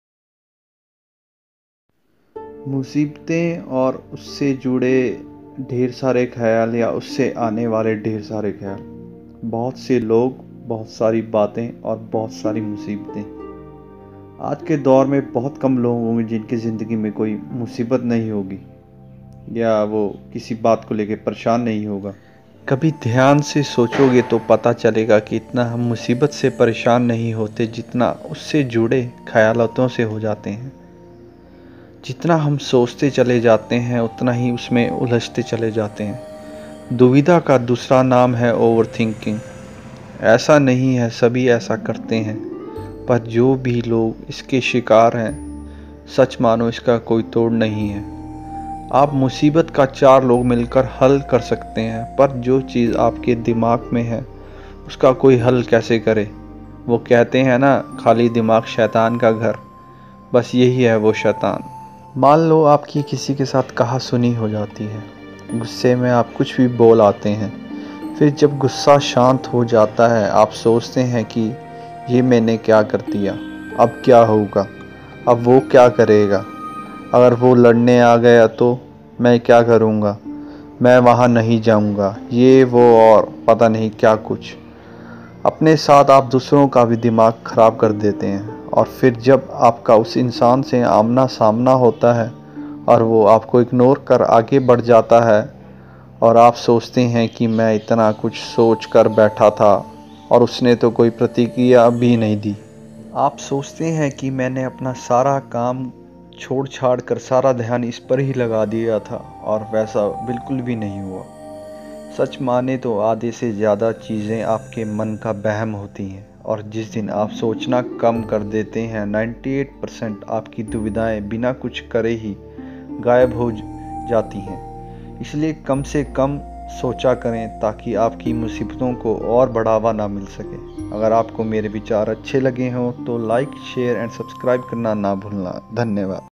Motivational videos || Problems and Overthinking ||Sharing Views||Lets talk||Let's Talk
In todays world everybody is facing problems in one way or other. And problem brings a lot of thoughts with it and we cal it overthing and its reallt kiliing human mind. max problems are mens self made . So stop creating your own problems and think less.